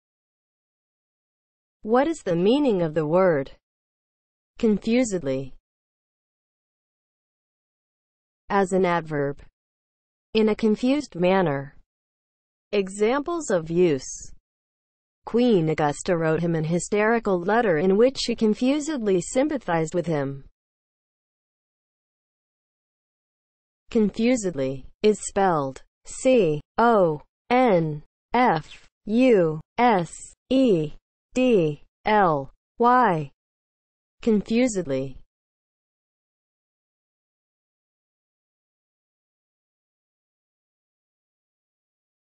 What is the meaning of the word CONFUSEDLY?
Definition, examples of use and spelling of the word CONFUSEDLY.

(00:00) Intro
(00:07) As an adverb
(00:22) Spelling

As an adverb (00:07):
• in a confused manner

Examples:
• "Queen Augusta wrote him an hysterical letter in which she confusedly sympathised with him"